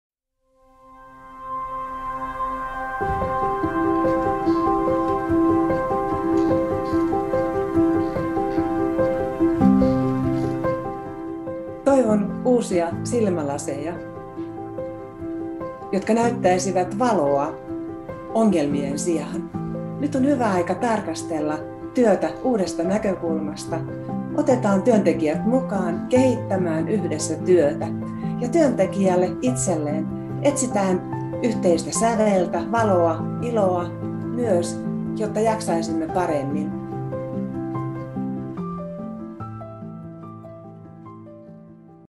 Wghjoulukalenteri - Marja Liisa Manka Luukku2
Wghjoulukanteri ssa työhyvinvoinnin asiantuntijat vastaavat kysmykseen

Mikä on työelämätoiveesi vuodelle 2022?